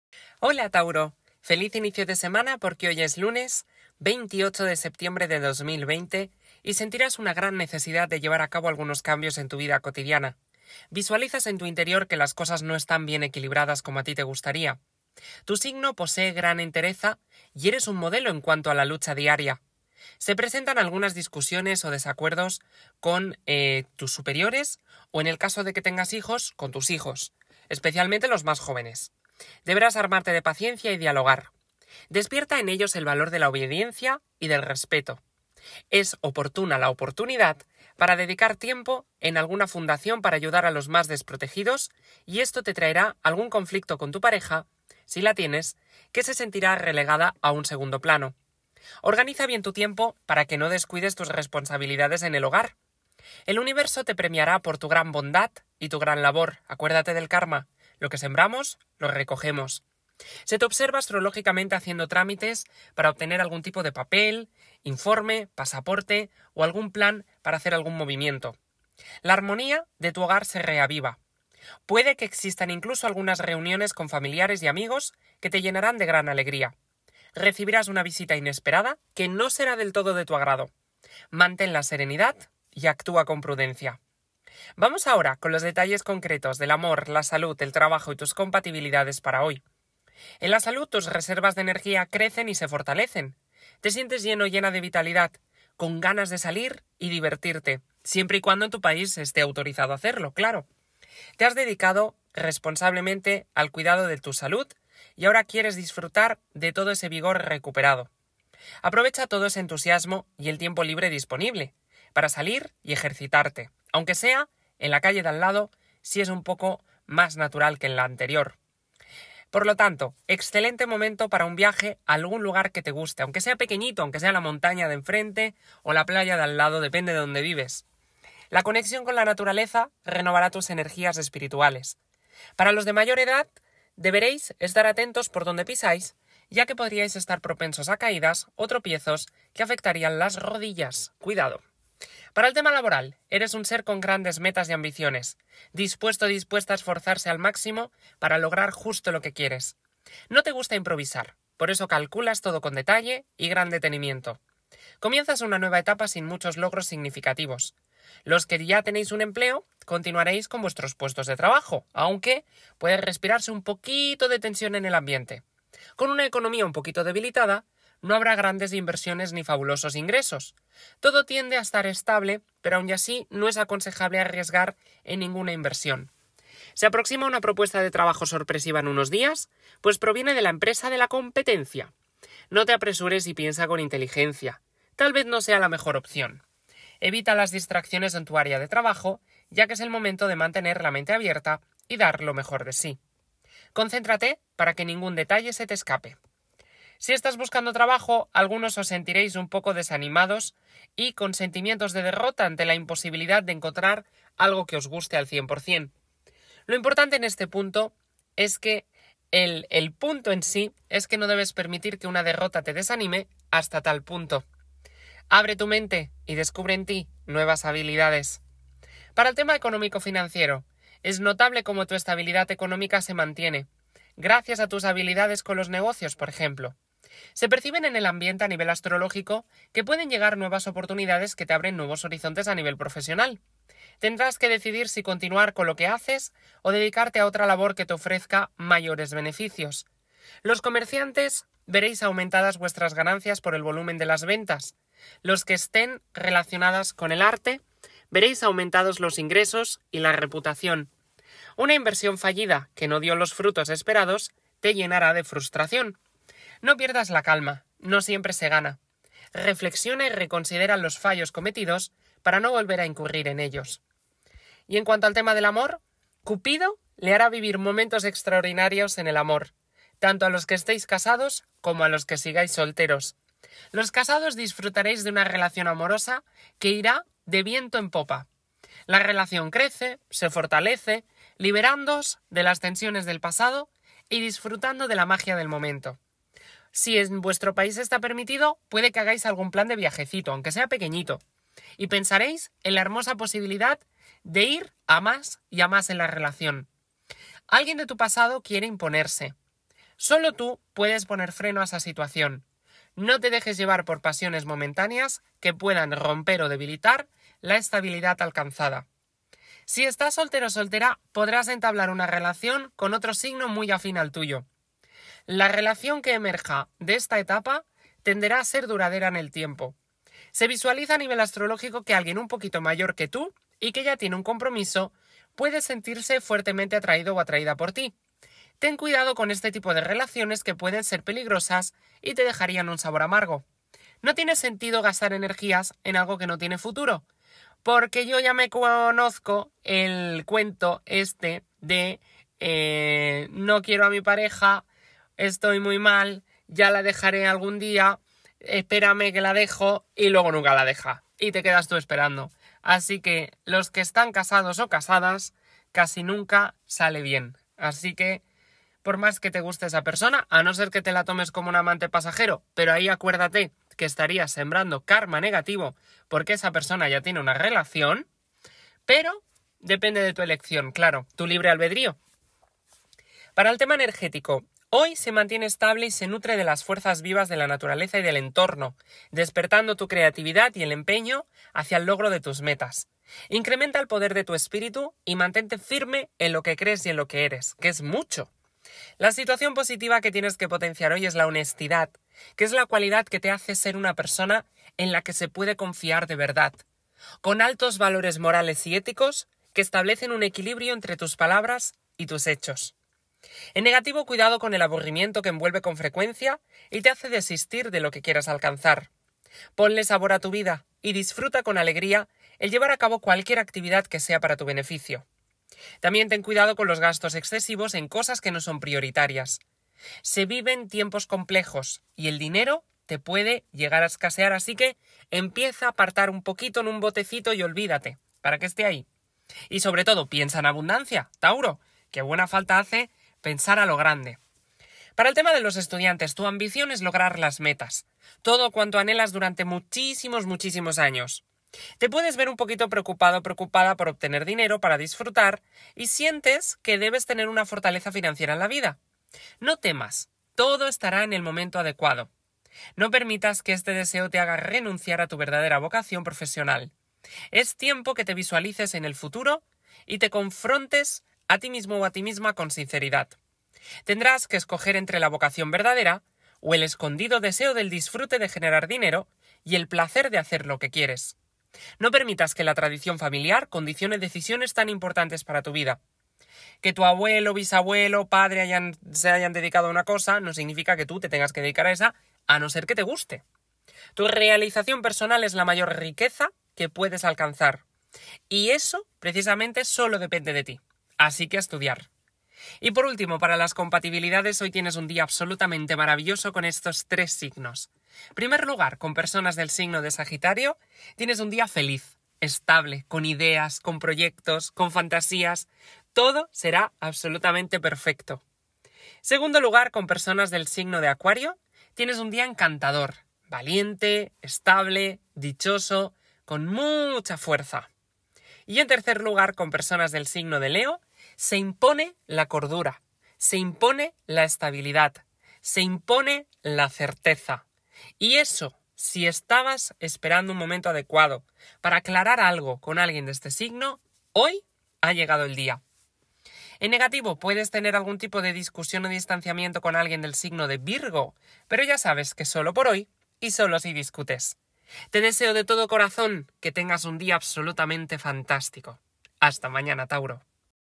Horóscopo Diario - Tauro - 28 de Septiembre de 2020
Horóscopo de hoy tauro 28 de Septiembre de 2020. Horóscopo de tauro hoy. Descubre tu futuro con mi horóscopo diario para ti, mi querido signo tauro en el amor, el trabajo, la salud, el dinero y las compatibilidades con otros signos. Horóscopo diario gratis!